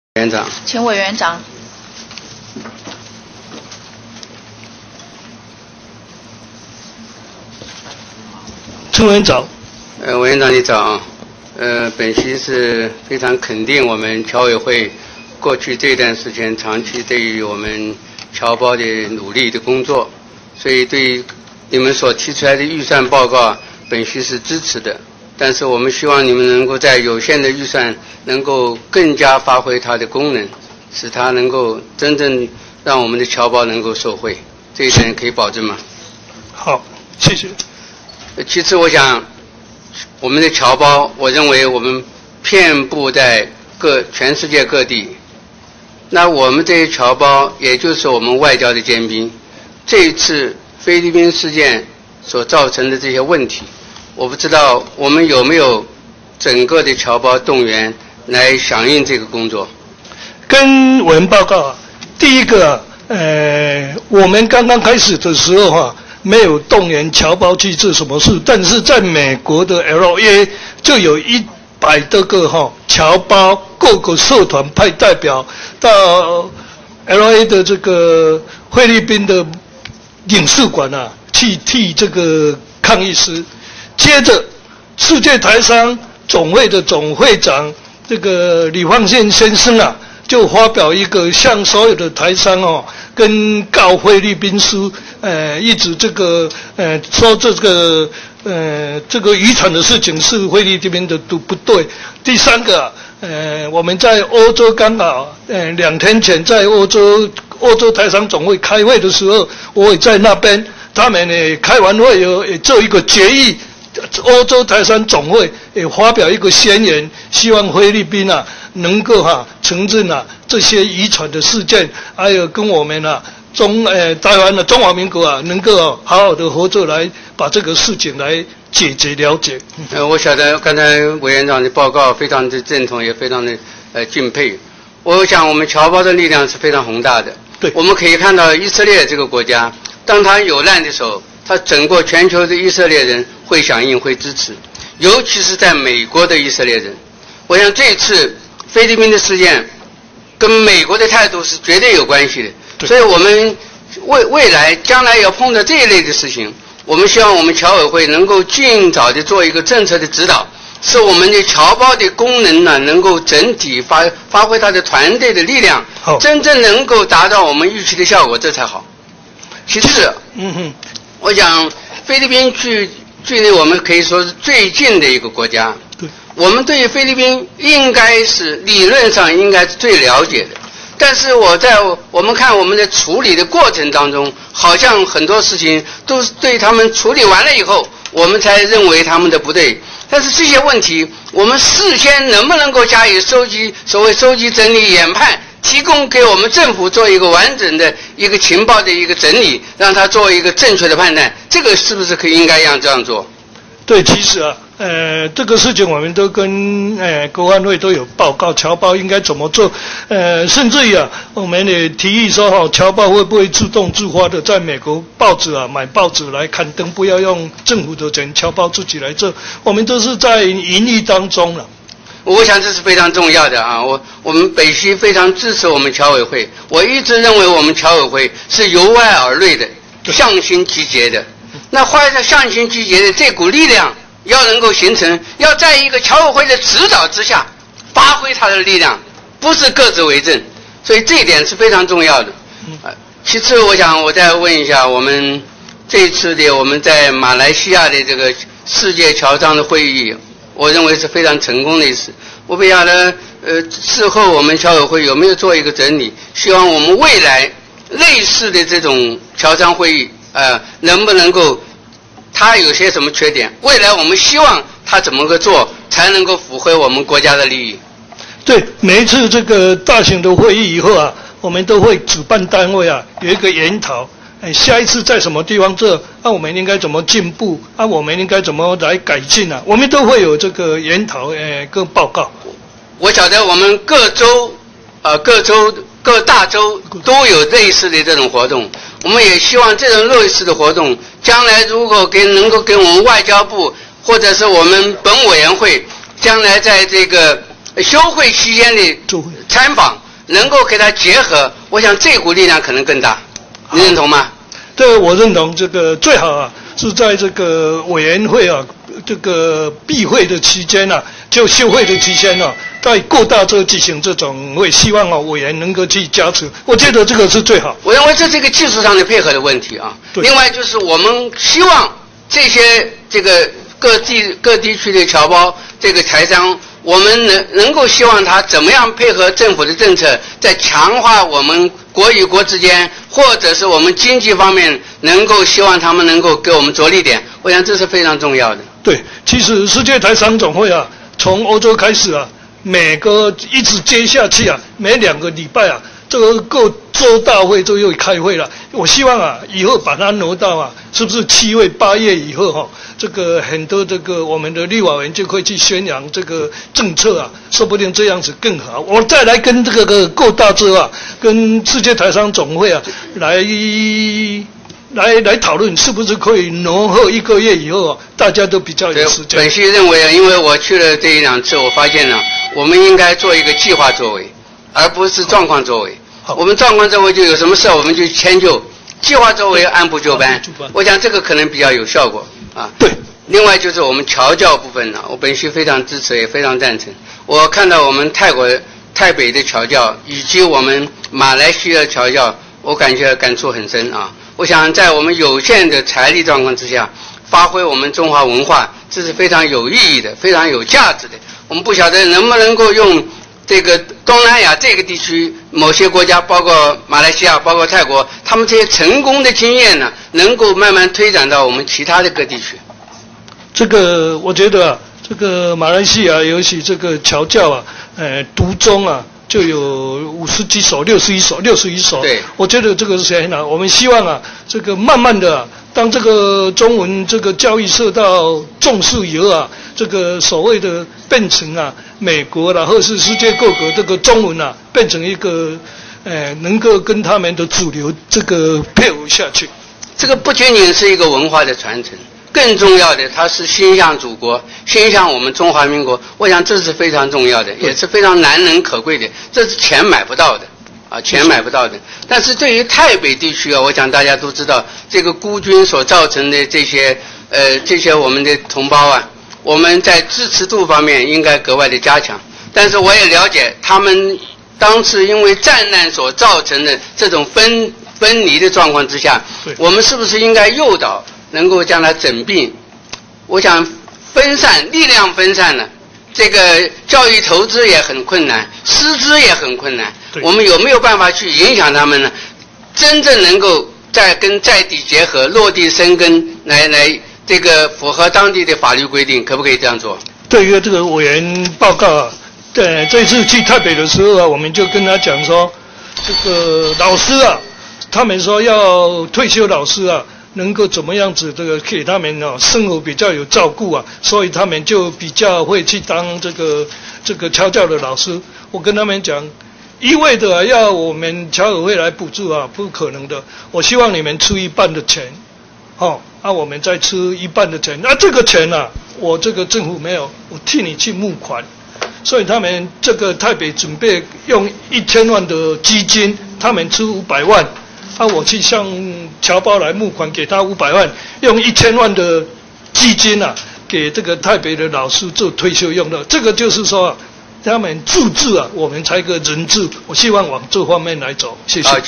2013-05-22 陳鎮湘 發言片段, 第8屆第3會期外交及國防委員會第27次全體委員會
會議簡介: 立法院第8屆第3會期外交及國防委員會第27次全體委員會議處理院會交付僑務委員會102年度預算凍結案等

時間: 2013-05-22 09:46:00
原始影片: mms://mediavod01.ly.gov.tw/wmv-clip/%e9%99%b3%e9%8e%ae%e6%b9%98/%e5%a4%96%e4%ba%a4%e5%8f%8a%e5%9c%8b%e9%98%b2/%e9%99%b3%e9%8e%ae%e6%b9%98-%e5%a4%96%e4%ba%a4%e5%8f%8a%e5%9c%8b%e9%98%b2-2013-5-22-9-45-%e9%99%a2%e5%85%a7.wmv